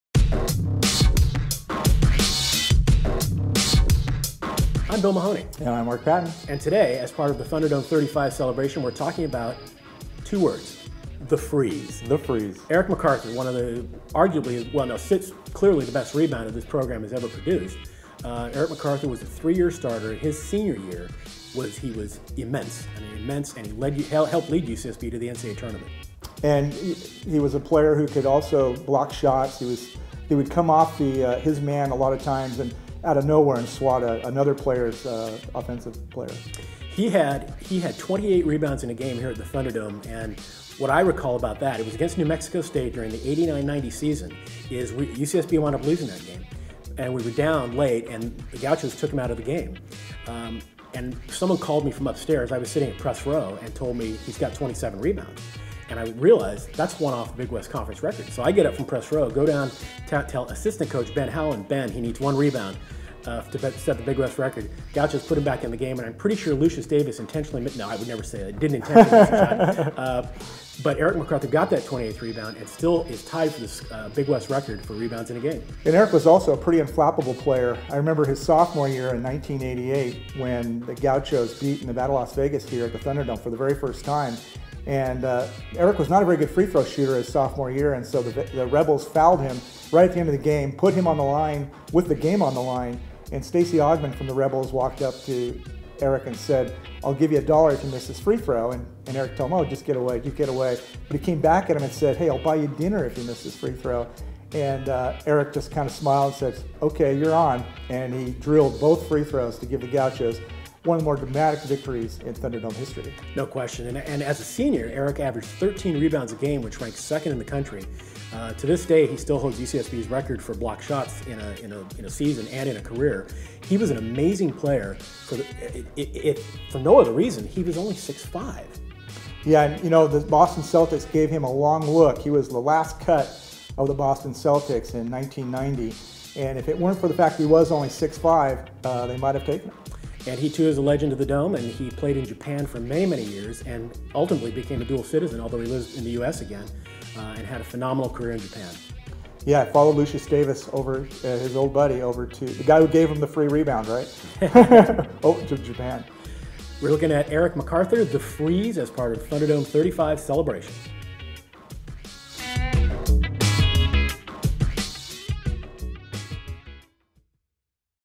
thunderdome35 - A Celebration of the Thunderdome - Eric McArthur
In honor of the 35th anniversary of the Thunderdome, once known as the Events Center, UCSB Athletics is honoring former Gaucho basketball legends who have played underneath its wooden ceiling.